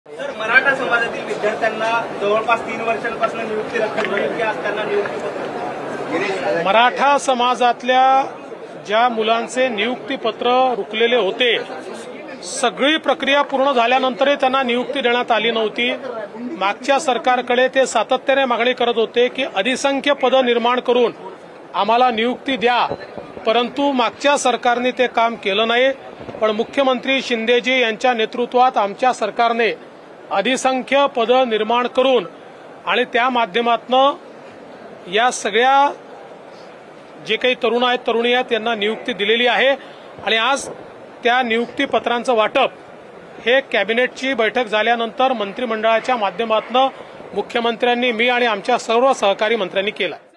प्रलंबित मराठा व इतर प्रवर्गातील 1064 उमेदवारांना नियुक्ती पत्र| मुंबई | DCM Devendra Fadnavis
प्रलंबित मराठा व इतर प्रवर्गातील 1064 उमेदवारांना नियुक्ती पत्र| मुंबई | DCM Devendra Fadnavis | 27 सप्टेंबर 2022